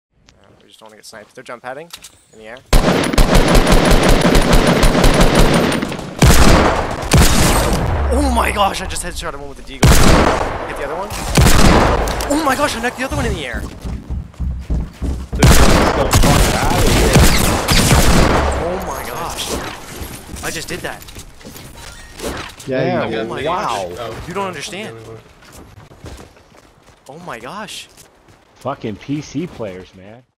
DOUBLE DEAGLE GLIDER KILL
I Snipe 2 enemies gliding in our base with the Deagle. I am still in shock Hope you guys enjoy!